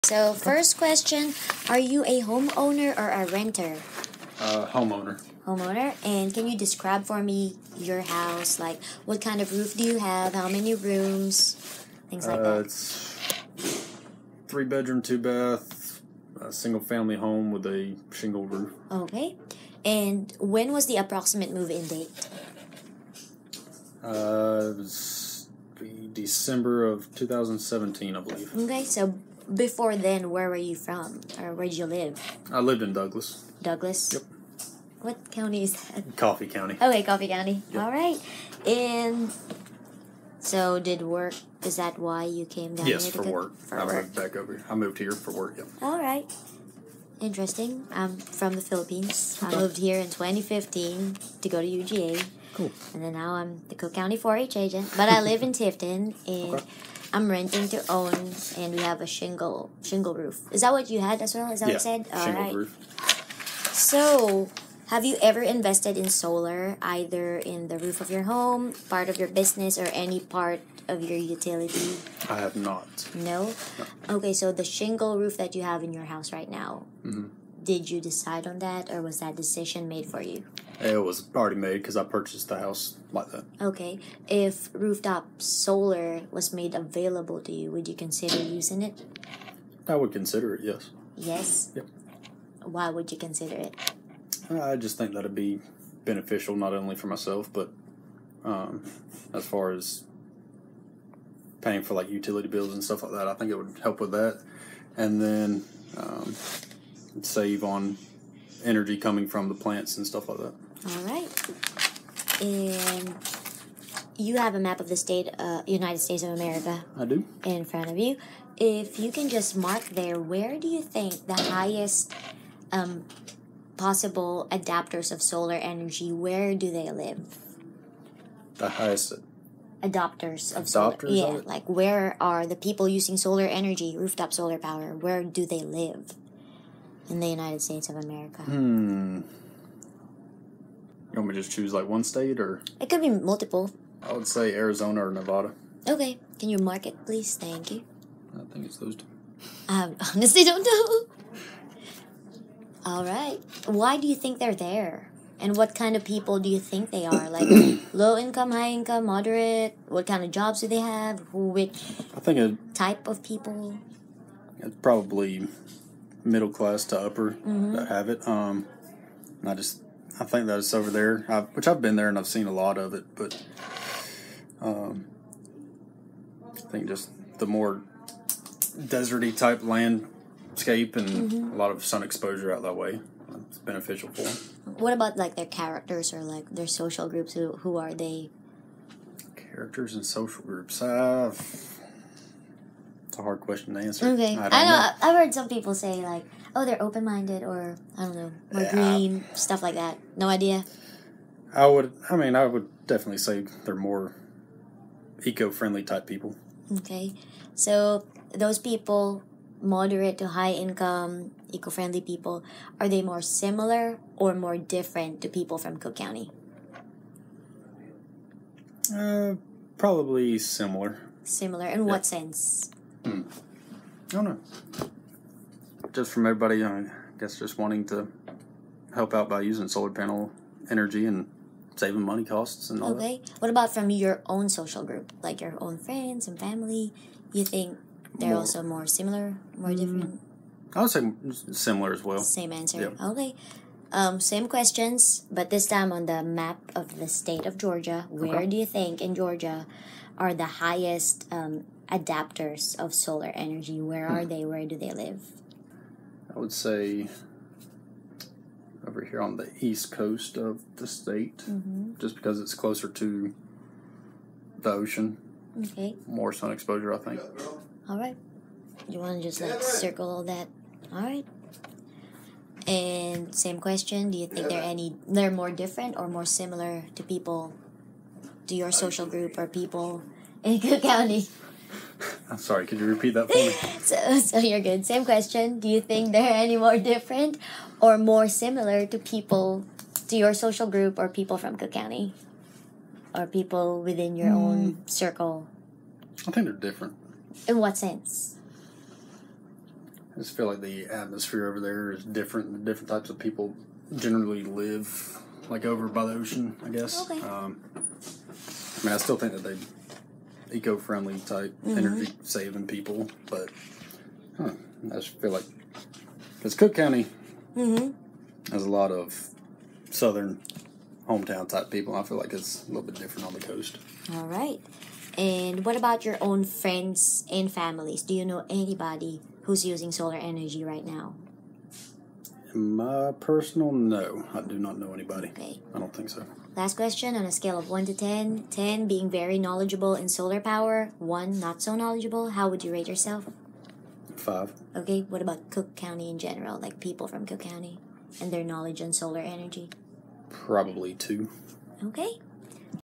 SolarSMART Interview - SEA-COOK-INF20
SolarSMART Interview conducted on 6/27/2019 by a UGA Cooperative Extension Service Agent with a 51 year old white male, homeowner from Cook County, Georgia.